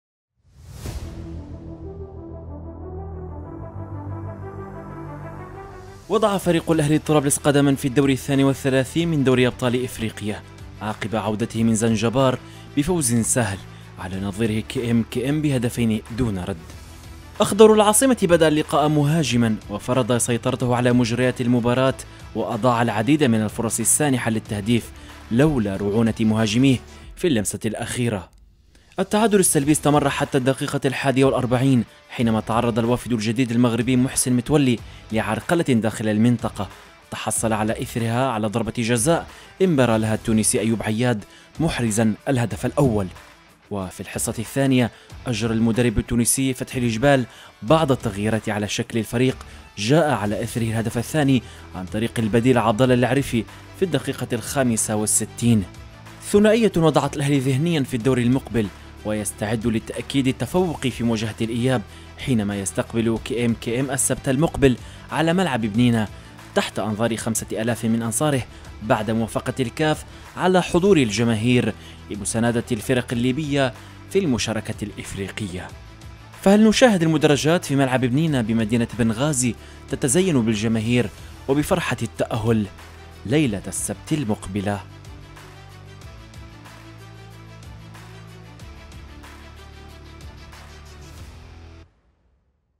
تقرير - 2 - 5 - 3 | الأهلي طرابلس يضع قدمًا في الدور الثاني والثلاثين من دوري أبطال إفريقيا
تقرير - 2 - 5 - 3 | الأهلي طرابلس يضع قدمًا في الدور الثاني والثلاثين من دوري أبطال إفريقيا عقب عودته بفوزٍ سهل من زنجبا

ــــــــــــــــــــــــــــــــــــــــــــــــــــــــــــــــــــــــــ
يمكنكم تحميل تطبيقنا TANASUH TV على الهواتف الذكية: